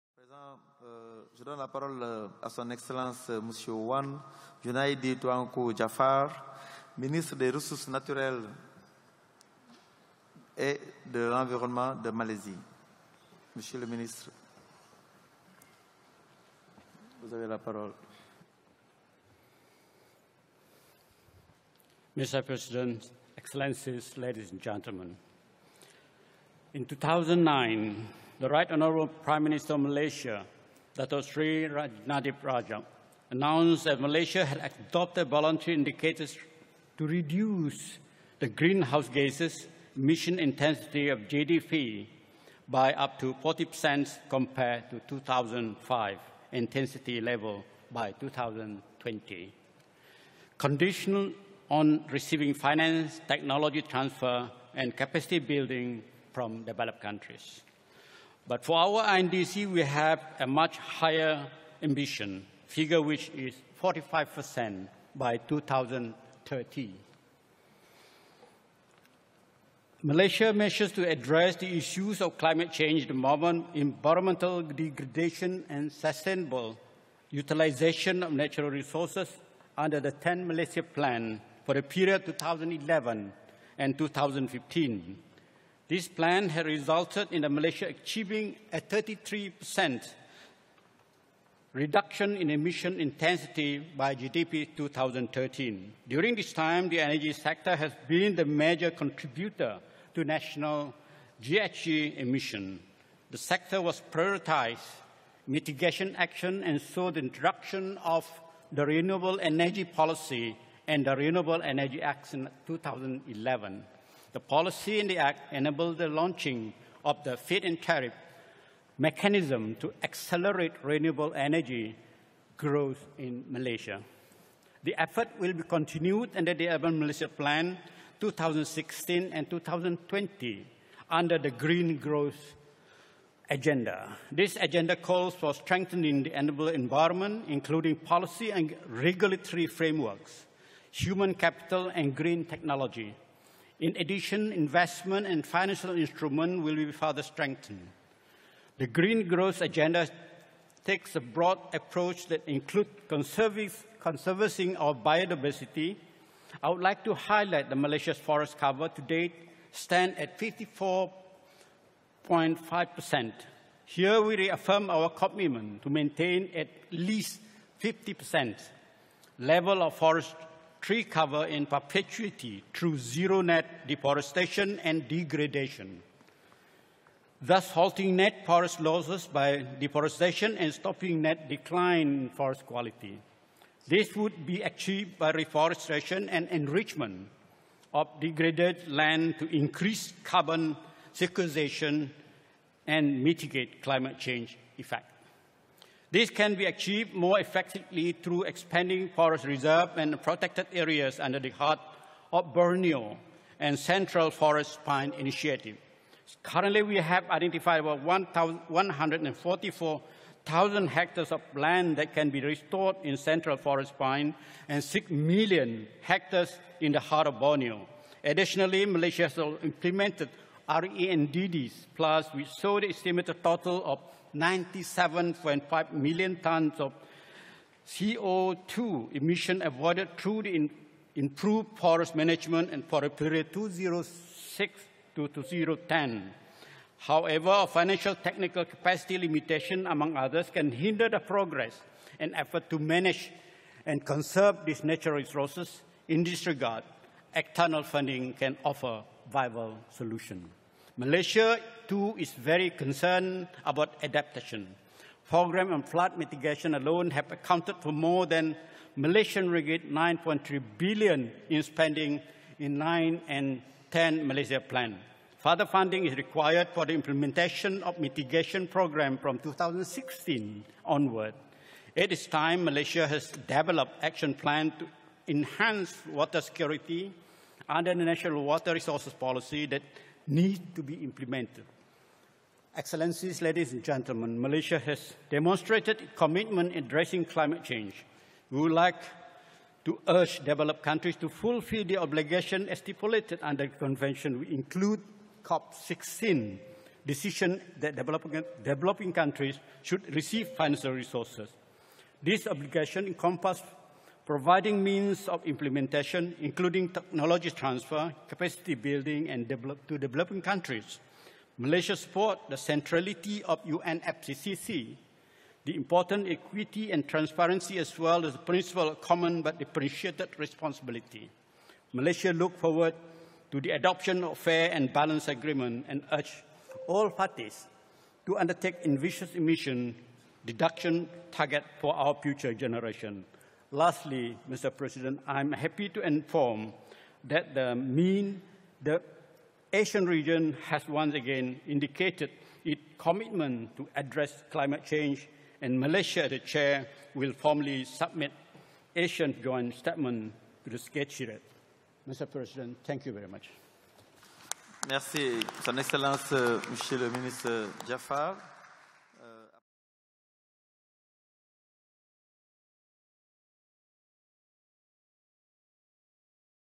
Datuk Seri Wan Junaid - Malaysia Statement COP21
HE DATO SRI DR WAN JUNAIDI TUANKU JAAFAR
HONOURABLE MINISTER OF NATURAL RESOURCES AND ENVIRONMENT MALAYSIA
FOR
JOINT HIGH LEVEL SEGMENT OF COP21/ CMP11